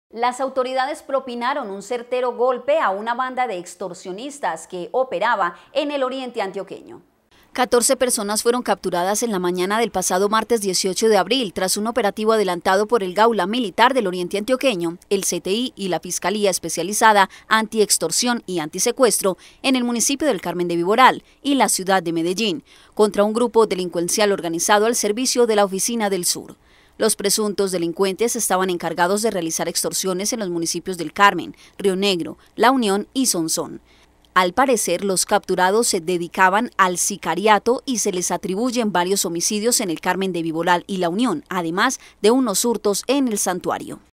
DURO GOLPE A LA EXTORSIÓN EN EL ORIENTE
Las autoridades propinaron un certero golpe a una banda de extorsionistas en el Oriente Antioqueño.